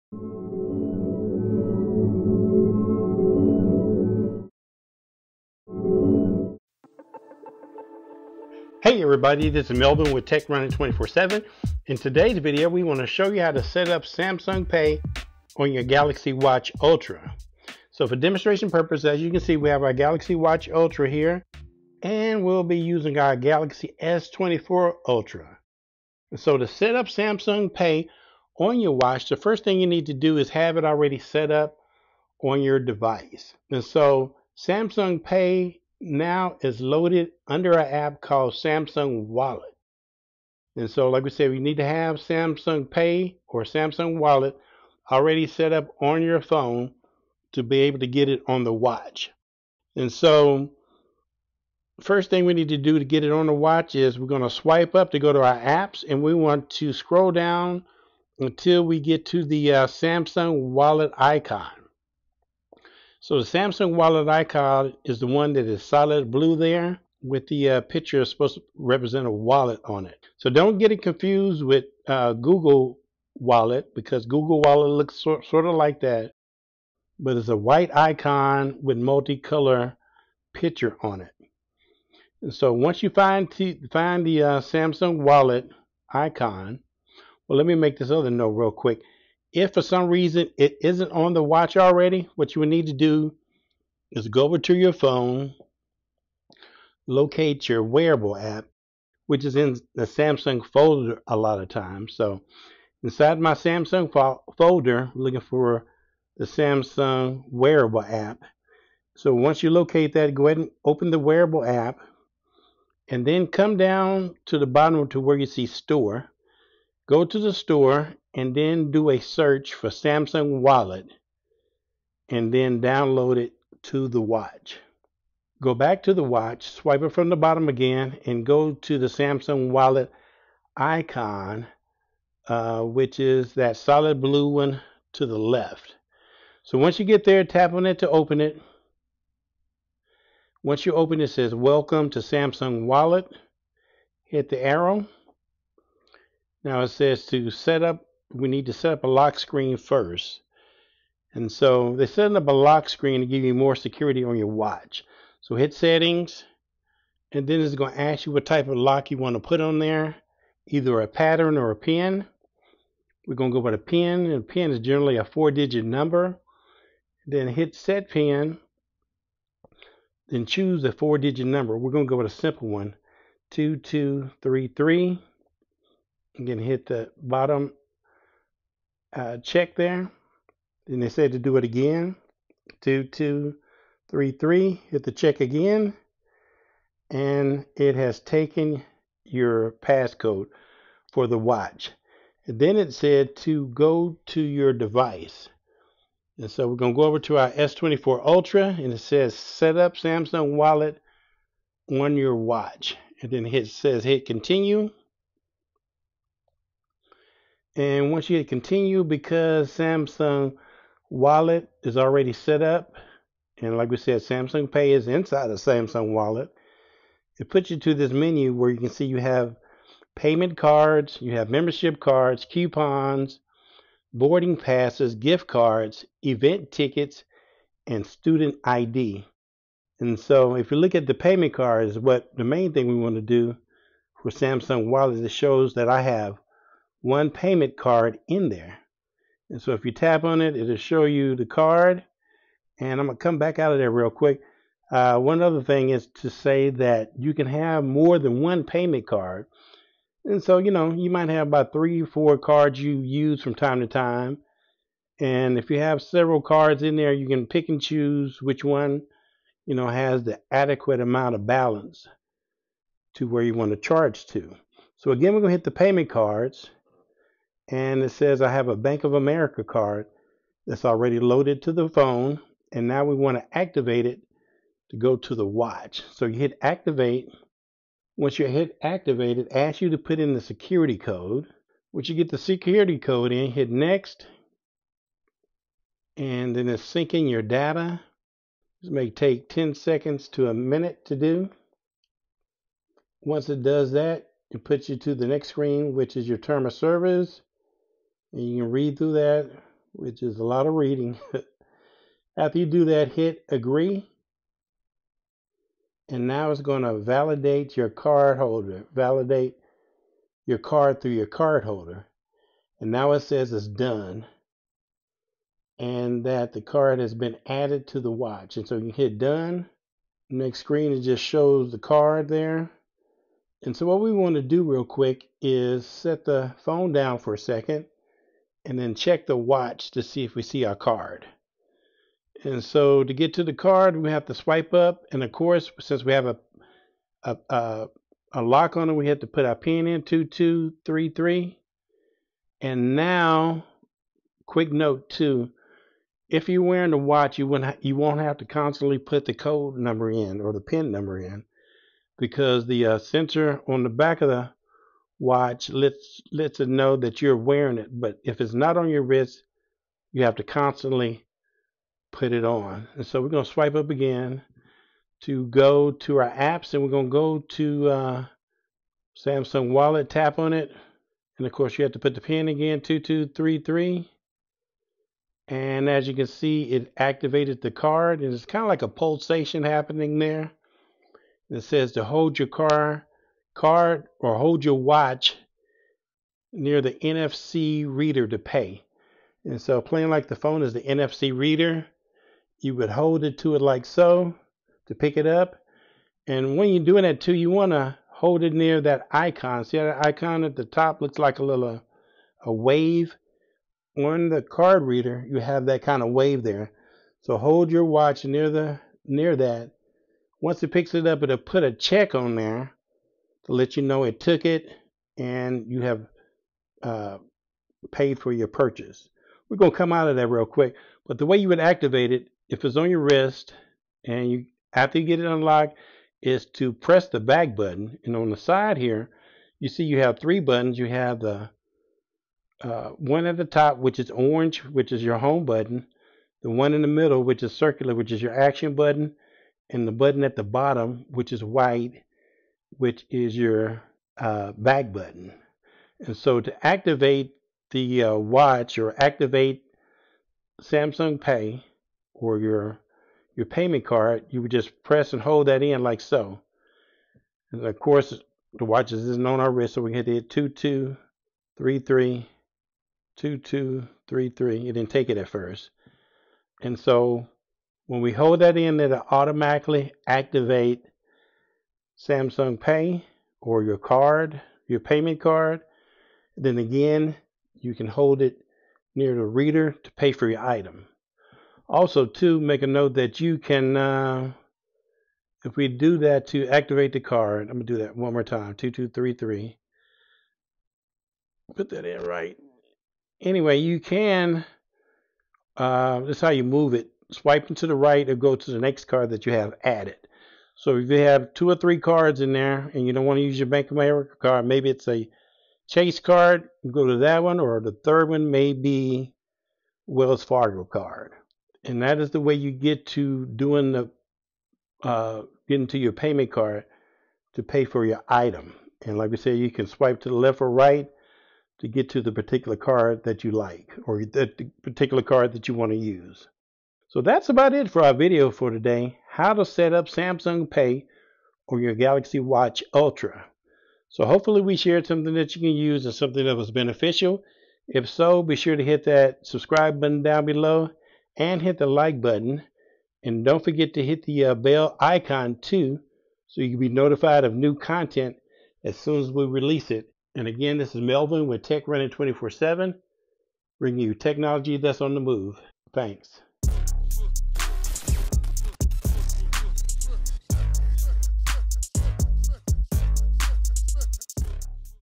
Setting Up Samsung Pay On Your Galaxy Watch Ultra: A Step-by-step Guide!
In this video we show you how to use and setup Samsung Pay on your Galaxy Watch Ultra.

Galaxy S24 Ultra

Galaxy Watch Ultra

3 in 1 MagSafe Wireless Charging Station